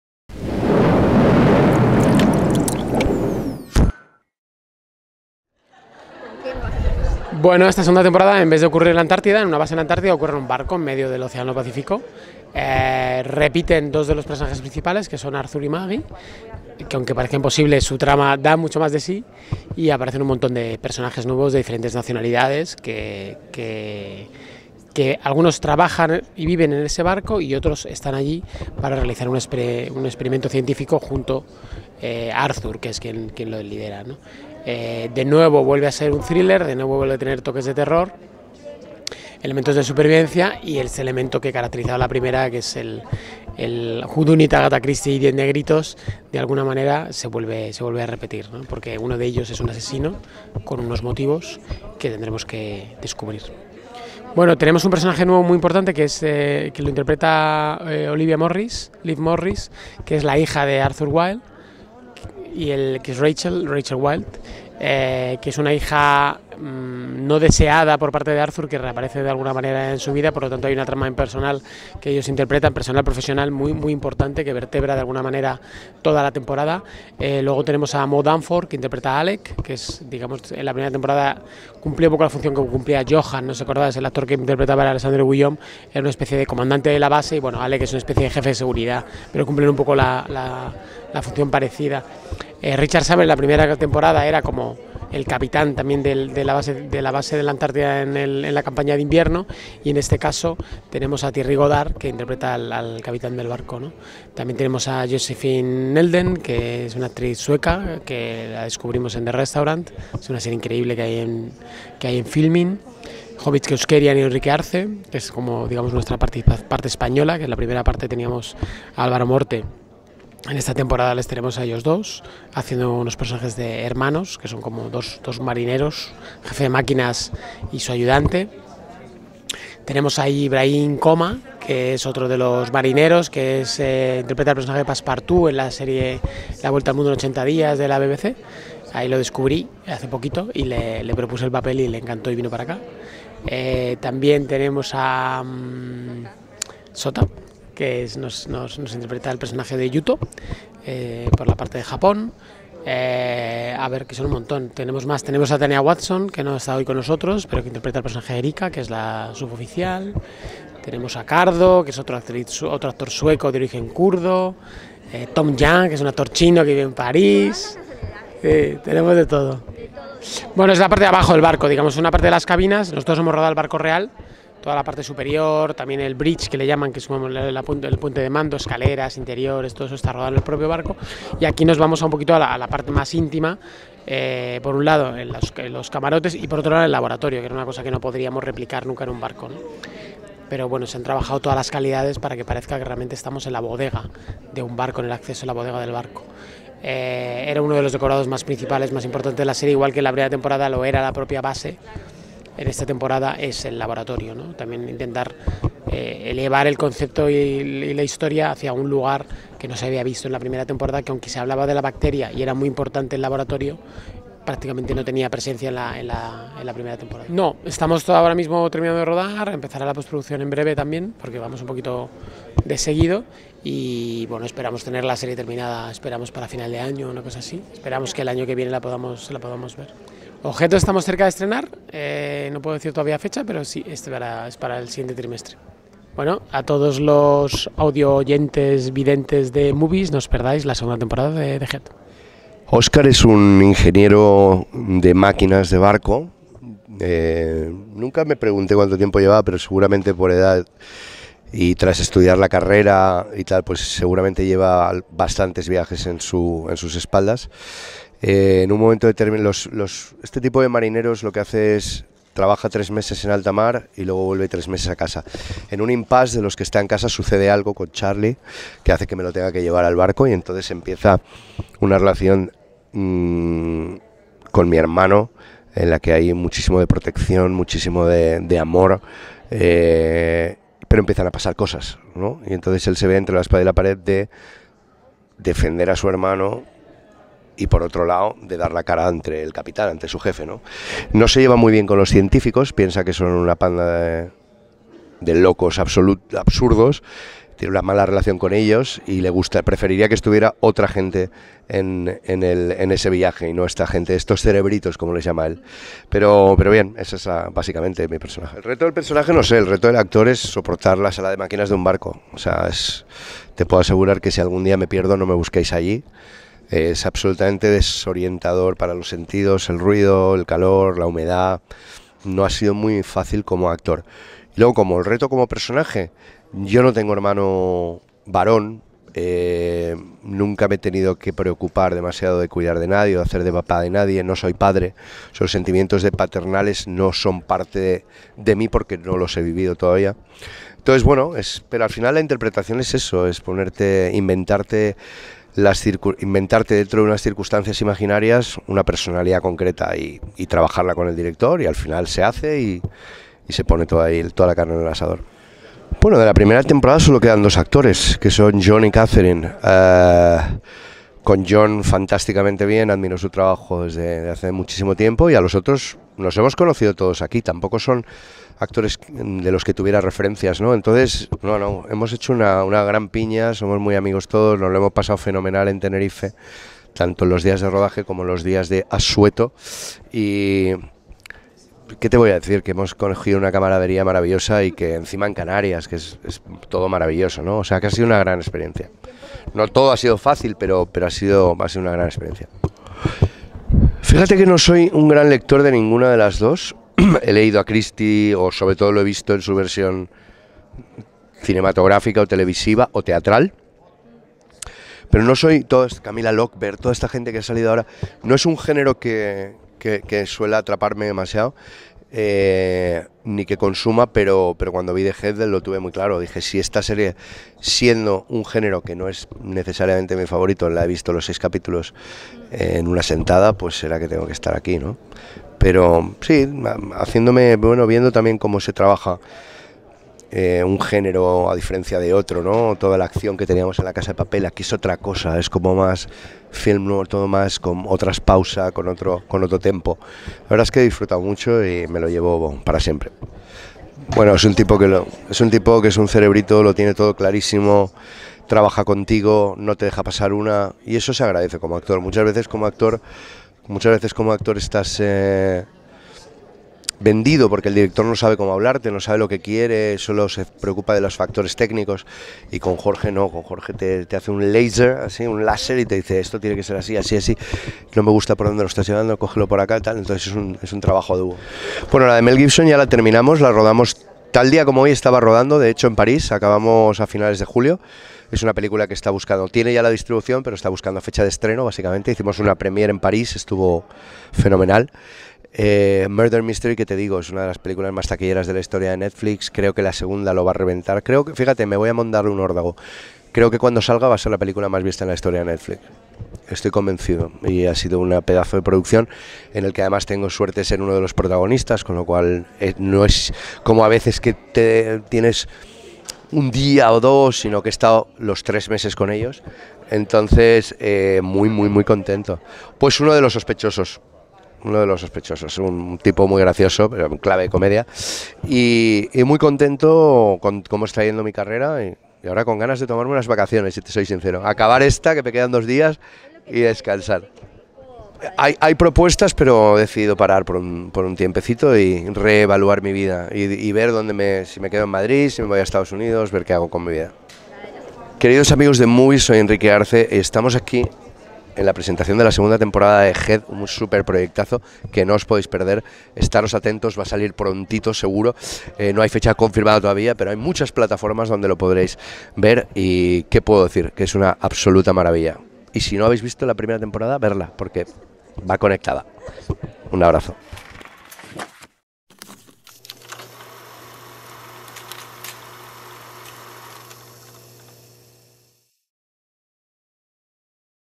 The Head 2 | Enrique Arce (La casa de papel) : "He decidido parar por un tiempo y reevaluar mi vida"
Hablamos con el director Jorge Dorado que estrenará este año la película "Objetos" con Álvaro Morte como protagonista y también charlamos con el actor Enrique Arce (Arturito en La casa de papel) que nos contó que está pensando irse a vivir a Estados Unidos... ¿qué pasará?